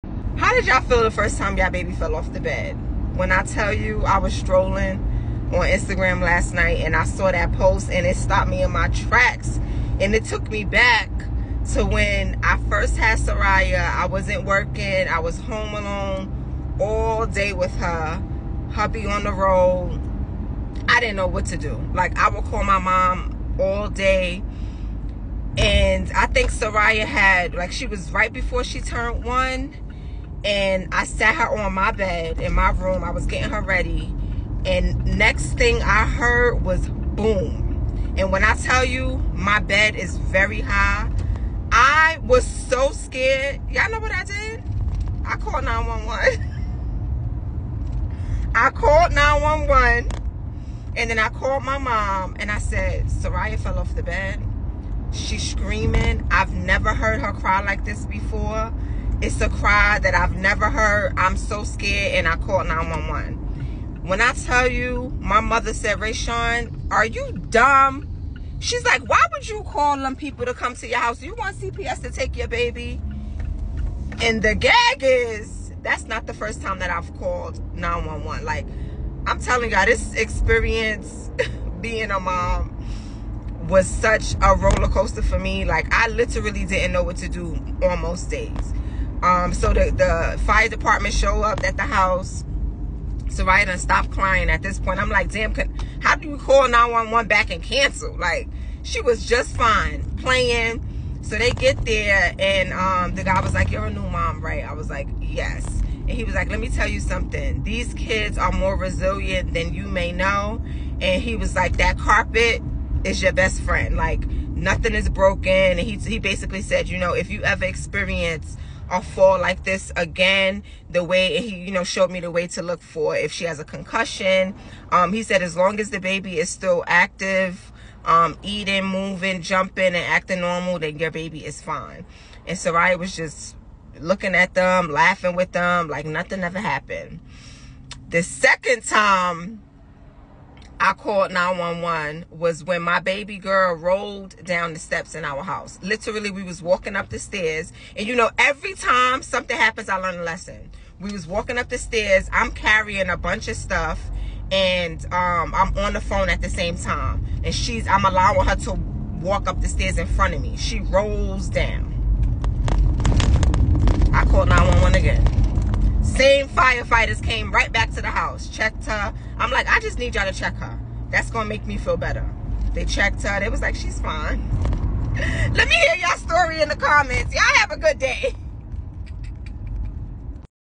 First-Time Mom: My Baby Fell Off the Bed! 😱 | Essential Tips for New Parents
In this video In this video, I share my personal experience of my baby’s first fall from the bed and the emotions that came with it. If you're a first-time mom, I’ve got you covered with essential tips on what to do in this stressful situation, including how to assess your baby’s condition, when to seek medical advice, and preventive measures to keep your little one safe. Don’t miss this important discussion to help you navigate the challenges of motherhood with confidence!